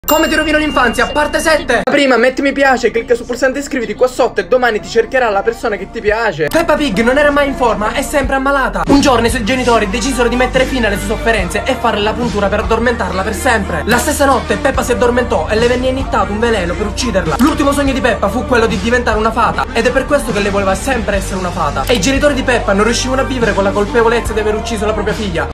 Come Ti ROVINO L'Infanzia 🥺
Ecco come ti ROVINO L'Infanzia in questo video. Questa è la verità dietro i cartoni animati di Peppa Pig! *TEORIA*

SU DI ME:
Bella raga, sono Jonny, quello che fa anche i video su TikTok.

Su questo canale troverai fatti interessanti, life hacks & storie incredibili!😱